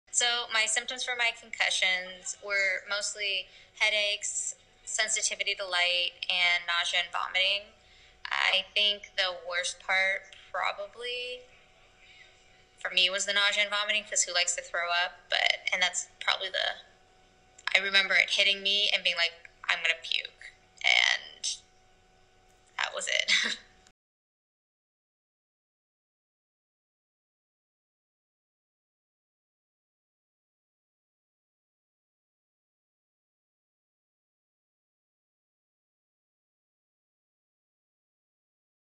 Emily's Field Hockey/Gymnastics Concussion Story: Physical or Mental Struggles
Emily talks about the "Physical or Mental Struggles" of the field hockey/gymnastics concussion story received at 16 years of age. To see all of her story and thousands of others visit the Concussion Story Wall

1) What was the cause of your concussion?
2) What were your symptoms? (Physical/mental struggles?)
3) What was the greatest challenge about your concussion?
4) What tips helped you get through your recovery process?
5) In retrospect, what is something helpful you could have said to yourself during recovery?
6) Were you comfortable sharing experiences about your concussion? Were your family, friends, team, coach, school helpful in this process?